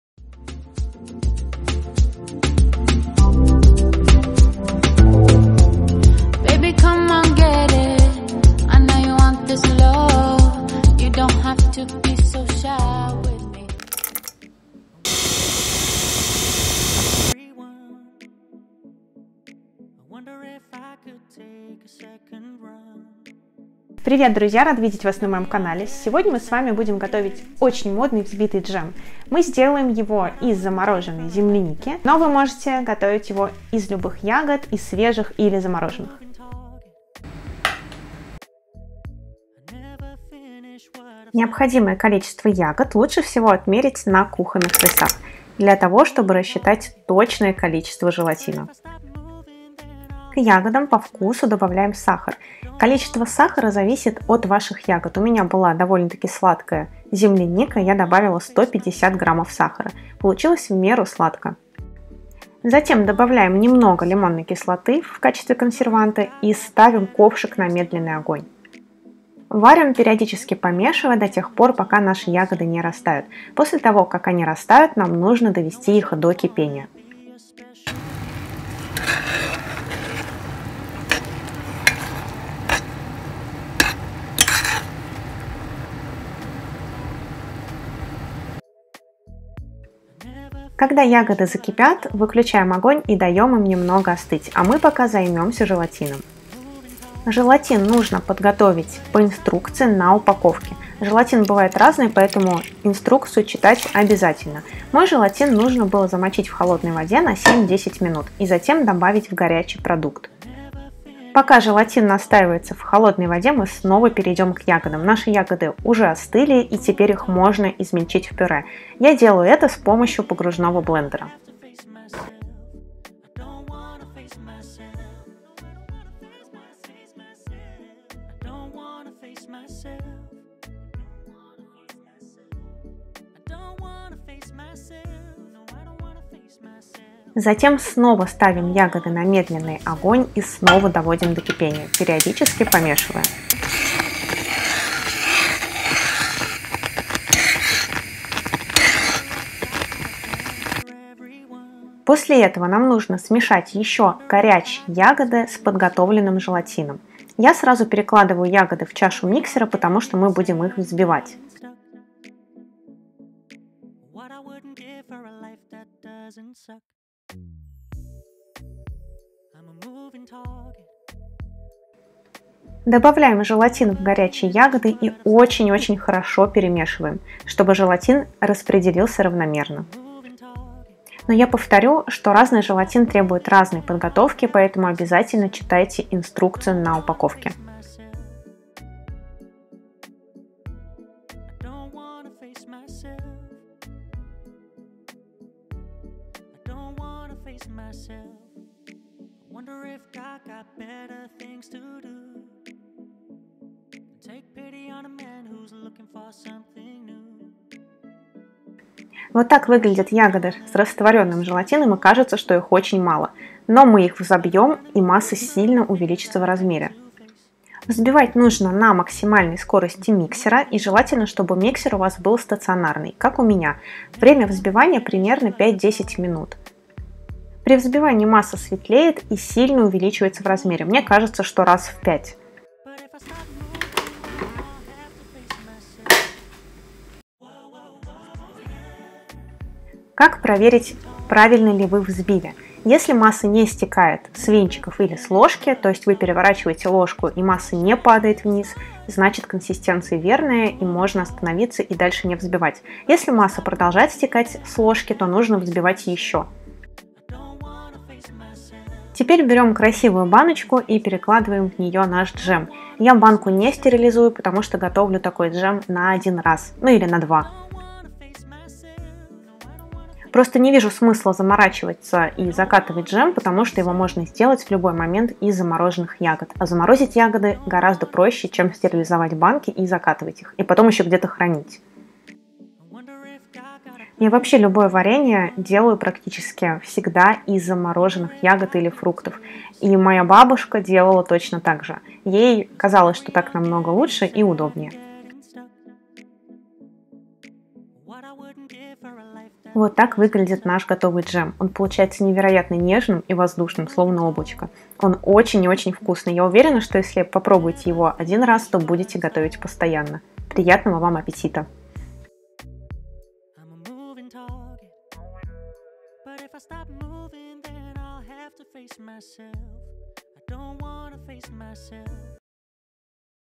Просто взбиваю варенье миксером и получаю невероятный воздушный десерт!
Взбитый джем или ягодный мусс (или ягодное суфле) – все это один рецепт. Очень вкусно и крайне быстро!

Что нужно:
ягоды – 500 г (у меня садовая земляника, которую все называют клубникой)
сахар – по вкусу, в зависимости от ягод. Я добавила 150 г и получилось в меру сладко
лимонная кислота – 0,5 ч.л.
желатин – 50 г (но нужно читать на упаковке – на моей пачке было написано, что рекомендуется добавлять 1 пластину на 100 г продукта)